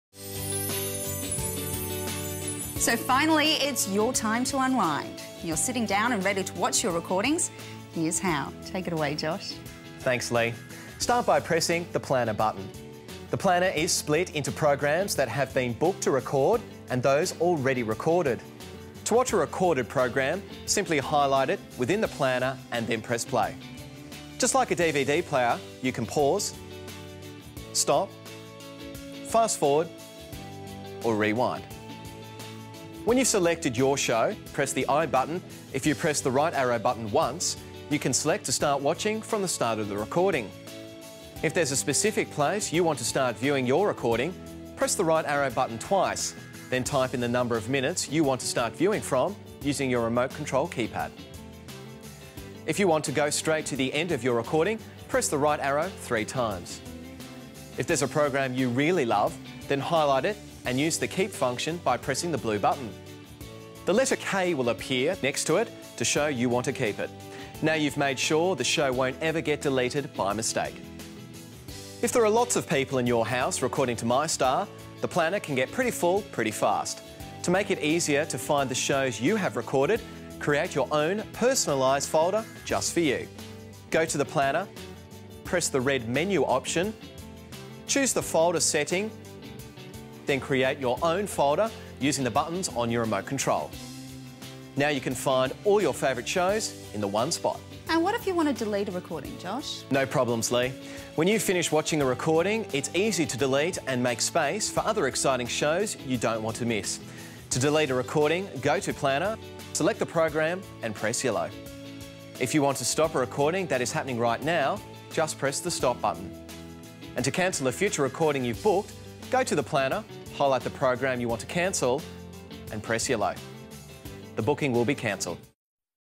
Manage your recordings in the Planner
Use the Planner in your MyStar to manage your MyStar recordings.